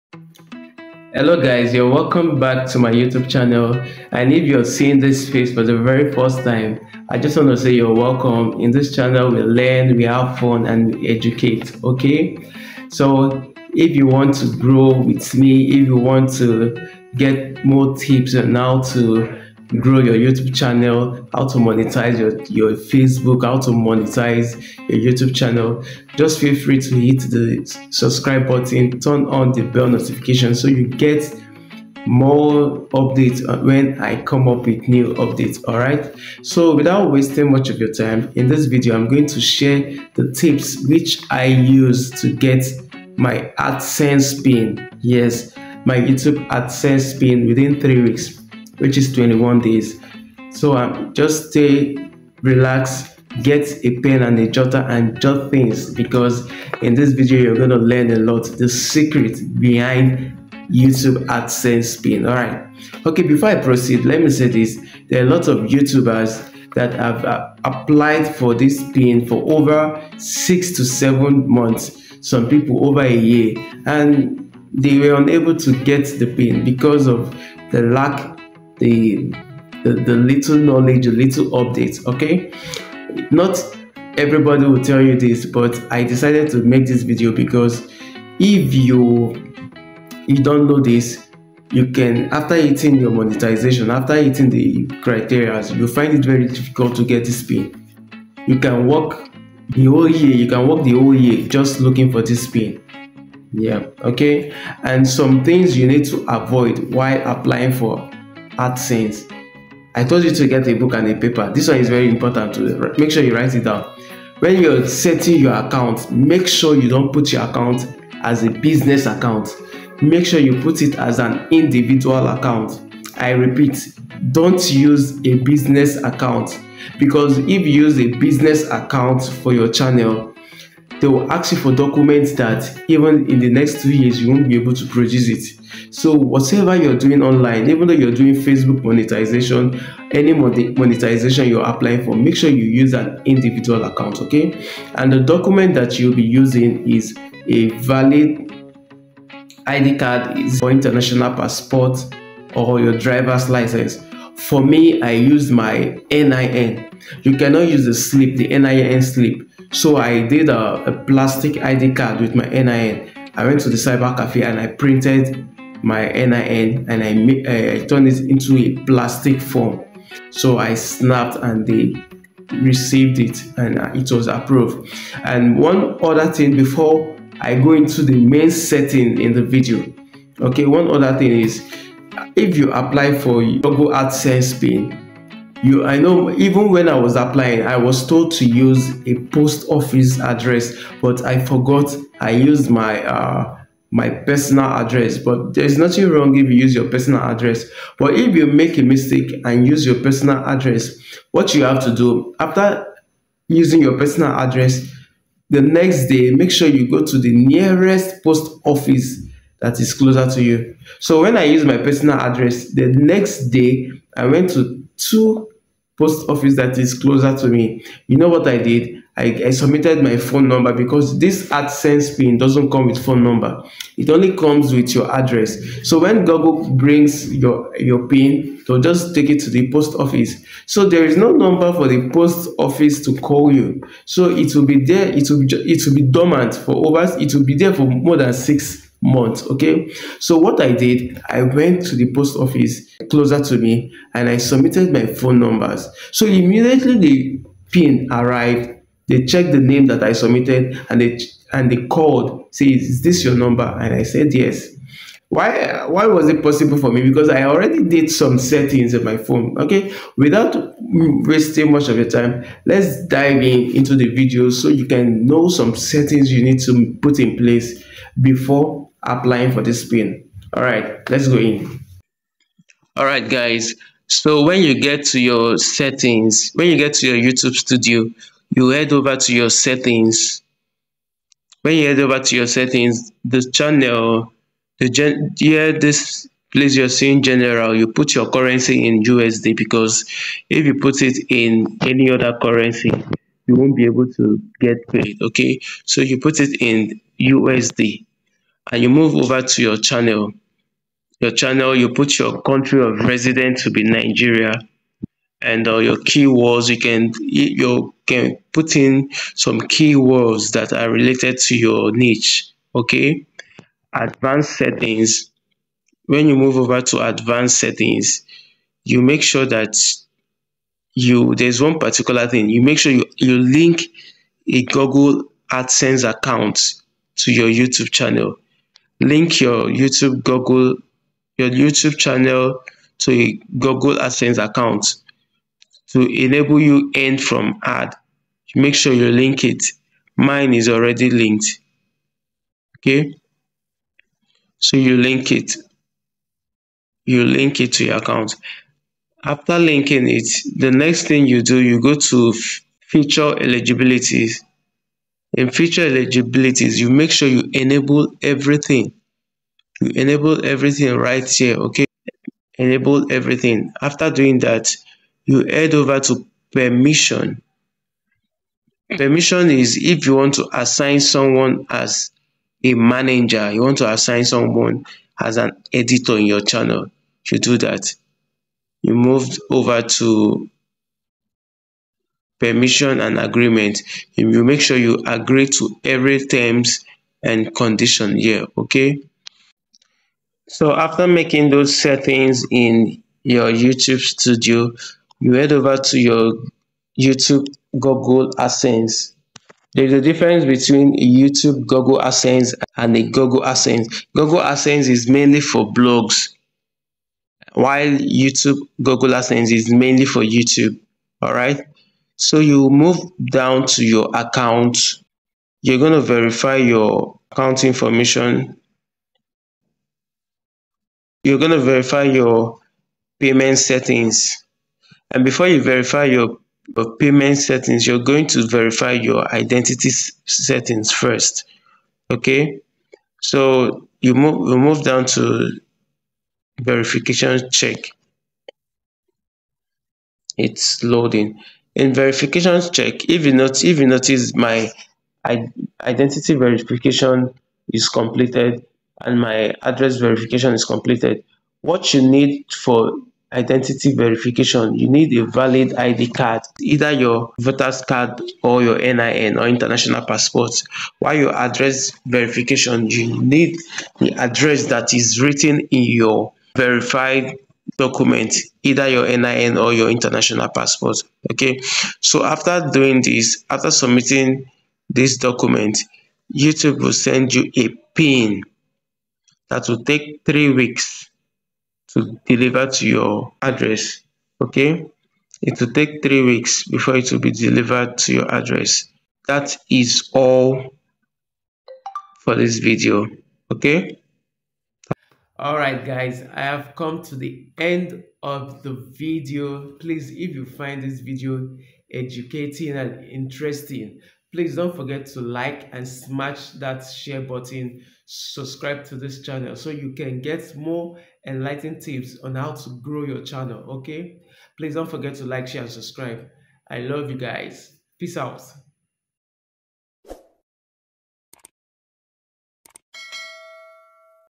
How To Verify Google AdSense Account And Get Your AdSense Pin Quickly
In this video, i will show you how i got my Google AdSense Pin quickly, without limitation.

Timestamps:
0:00 - Introduction
1:45- Things To Avoid Before Applying for Google AdSense
2:28 - Document for  verification
2:59 - How To Apply For Google AdSense
4:17 - Settings Before Applying For Google AdSense
5:25 - How To Link Google AdSense Account To You-tube Channel
6:40 - Difference Between Google AdSense and You-tube Google AdSense
7:10 -Identity Verification
7:41 - Address Verification
8:08 - Conclusion.